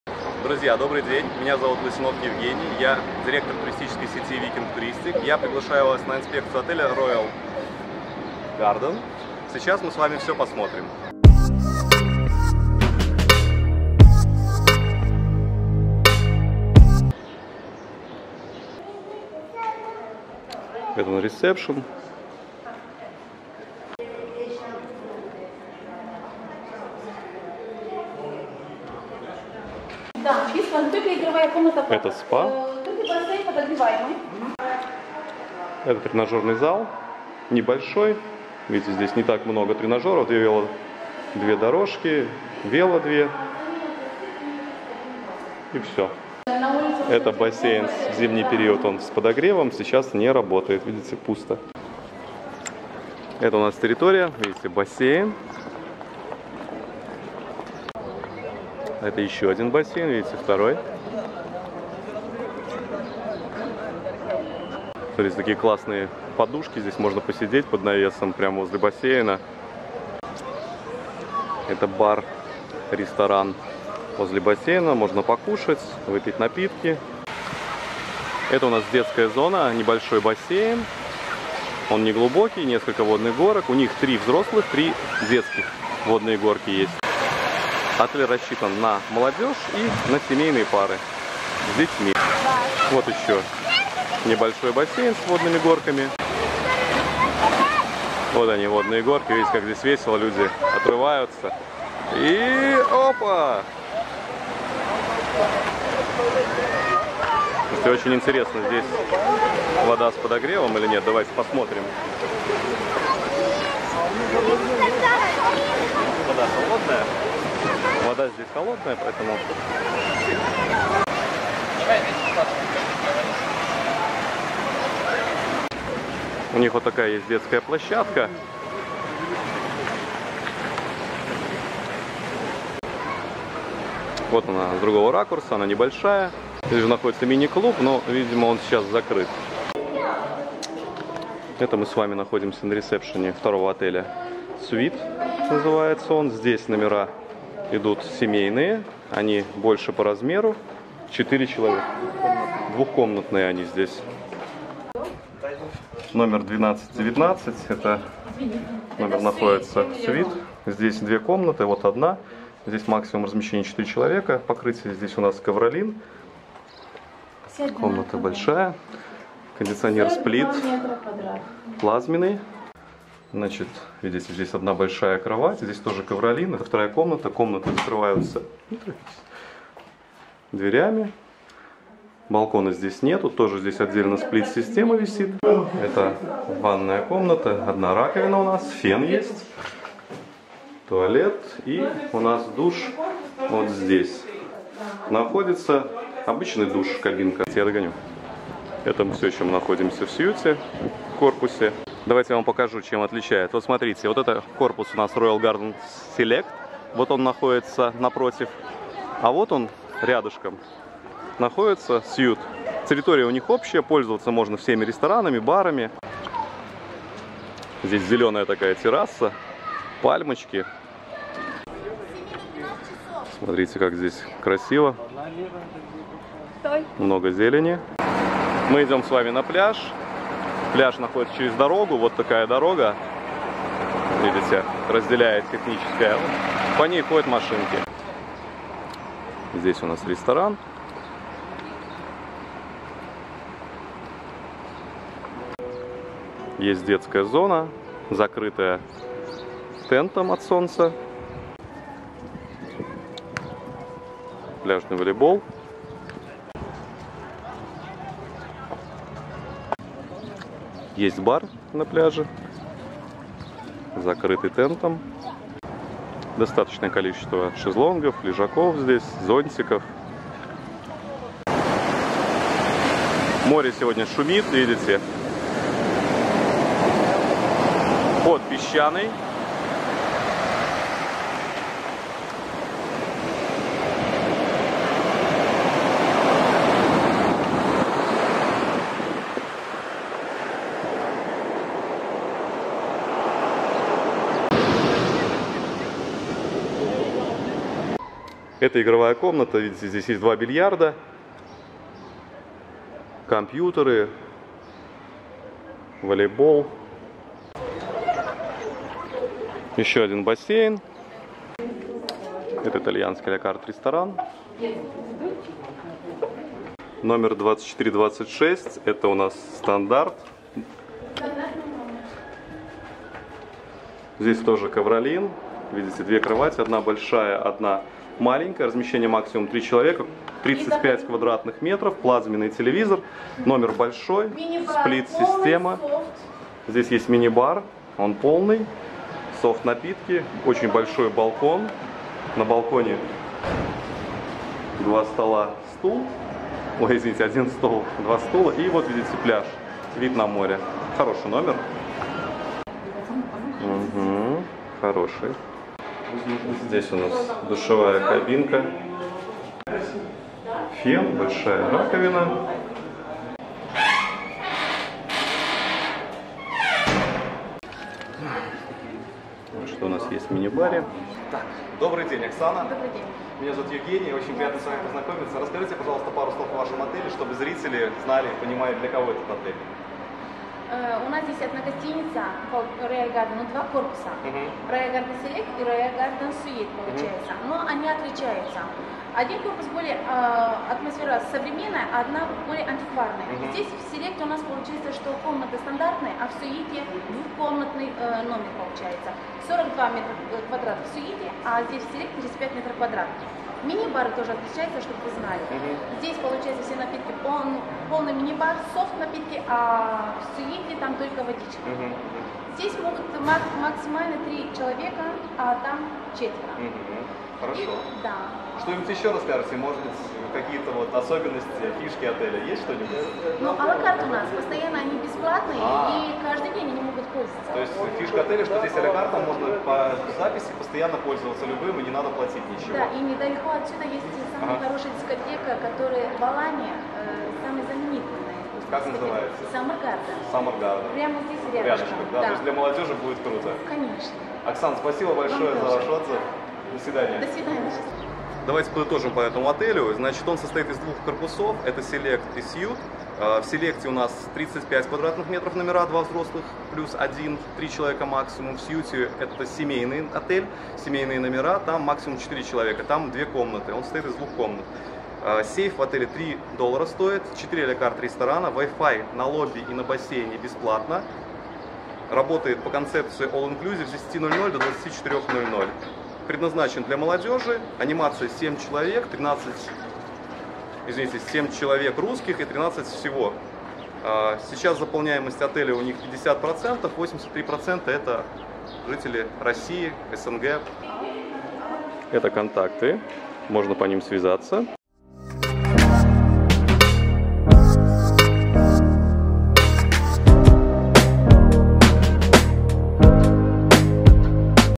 Обзор отеля Royal Garden Suite 4* (Роял Гарден Сьют), Турция, Аланья. 2018
Отель Royal Garden Suite состоит из двух зданий, предлагающих комфортабельное размещение и профессиональный сервис. Отдых обещает быть веселым и беззаботным, благодаря внимательному обслуживанию и и теплой гостеприимной атмосфере.
Отель расположен в поселке Конаклы, в 16 км от Алании, 110 км от аэропорта, в 120 км от Анталии.